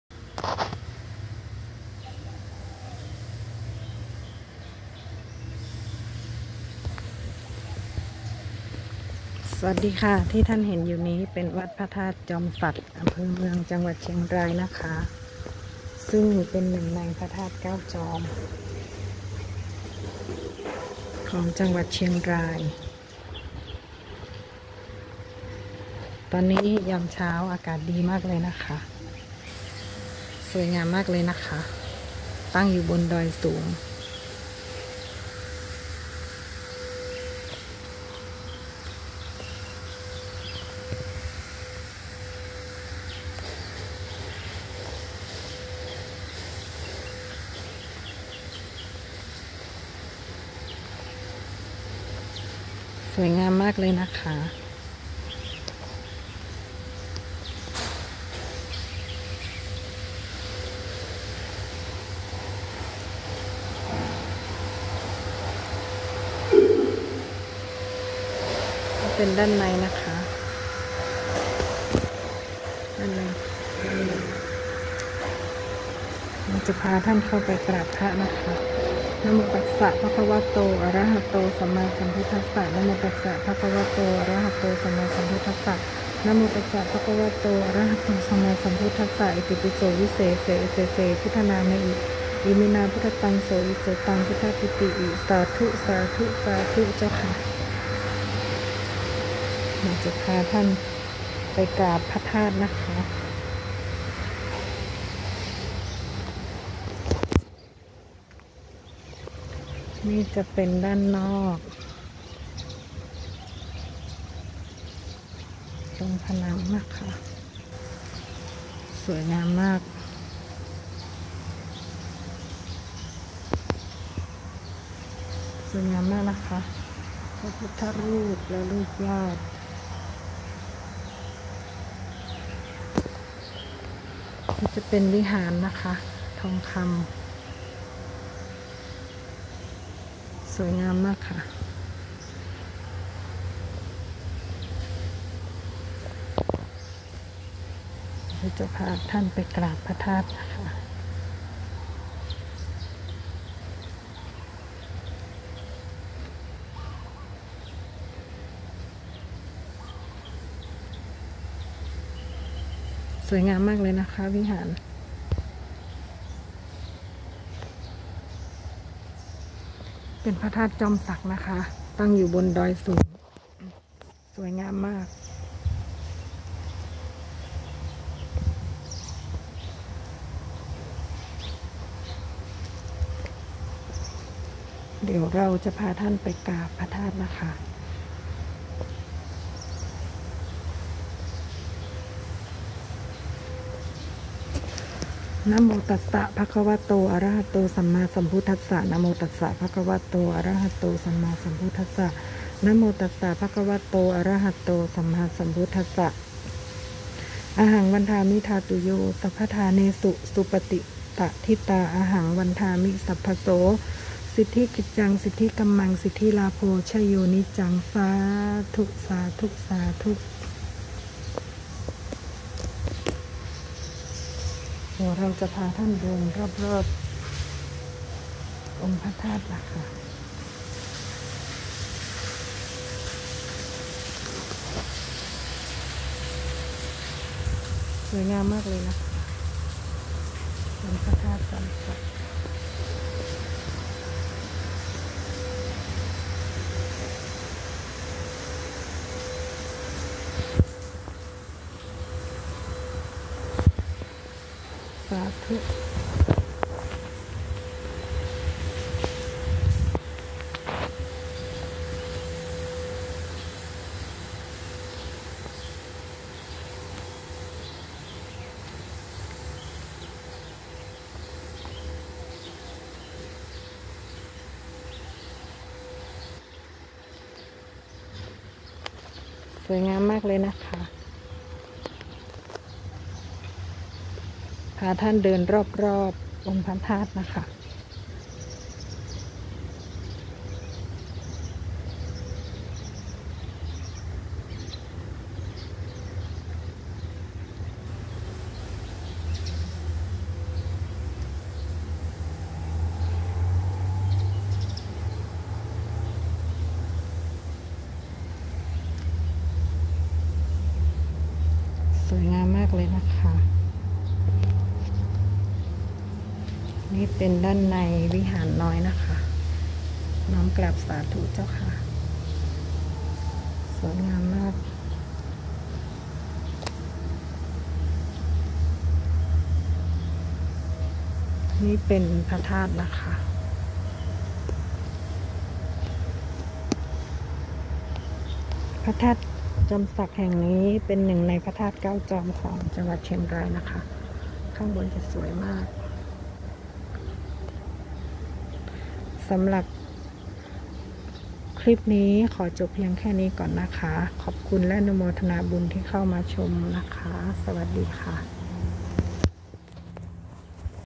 พระธาตุจอมสัก 1 ในพระธาตุ 9 จอมของเชียงราย
วัดพระธาตุจอมสัก เป็นหนึ่งในพระธาตุ9จอมของจังหวัดเชียงราย ตั้งอยู่ตำบลบ้านดู่ อำเภอเมือง จังหวัดเชียงราย ซึ่งมีความงดงามมาก